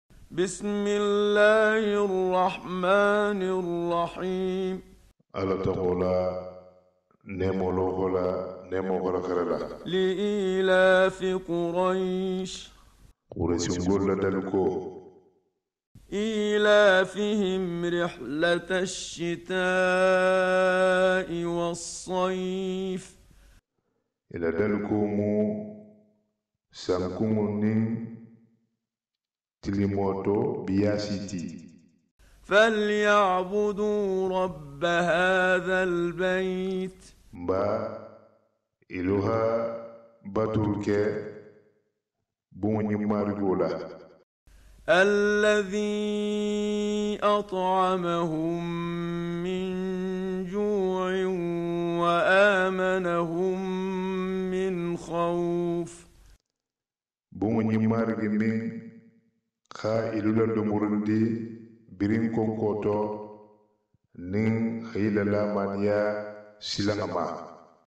LE Noble Coran en Diakhanké sourat Kouaish
traductions coranique  Afique de l'Ouest avec une precission grammaticale incryoyable